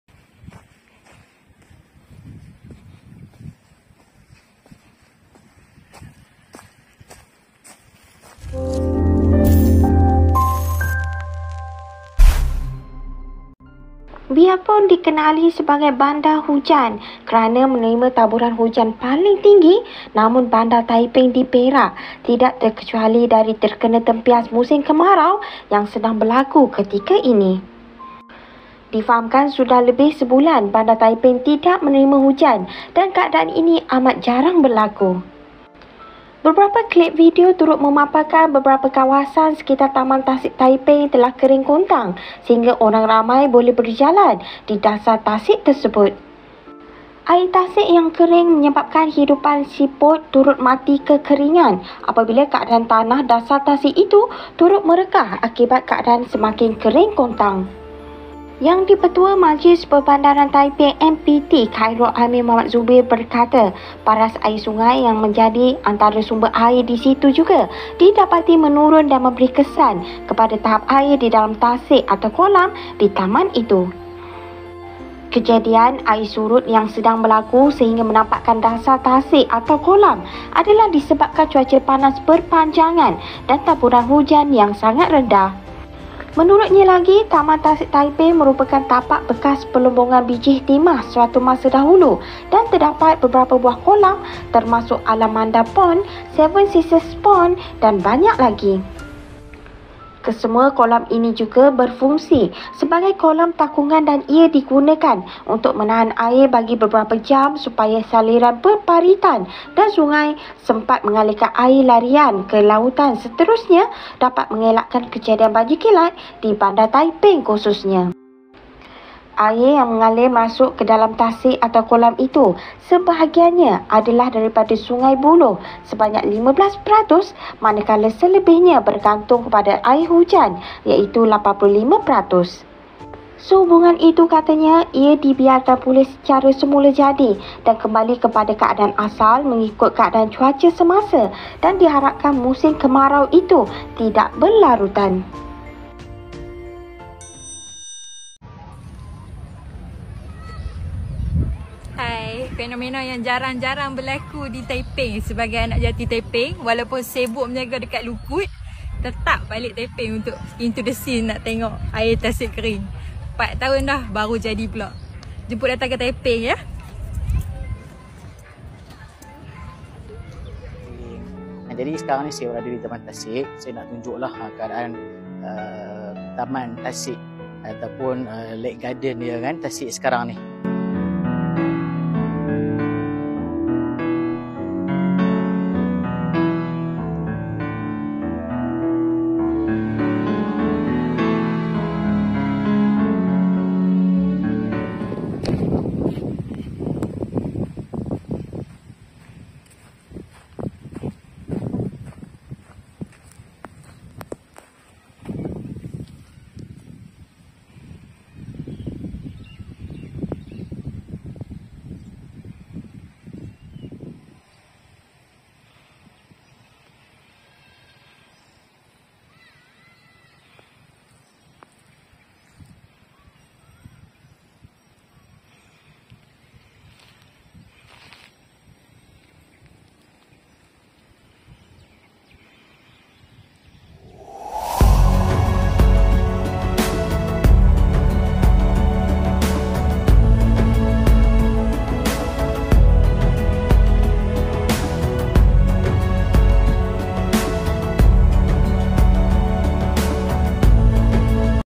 DAHSYAT‼️FENOMENA KEMARAU LUARBIASA DI TAMAN TASIK TAIPING, PERAK🔴"BANDAR HUJAN" KINI KERING KONTANG
Benda wajib ada  dalam kenderaan anda  :
70mai Dashcam Pro Plus+ A500S Car Recorder 1944P Rear Cam A500 Night Vision ADAS Parking Mode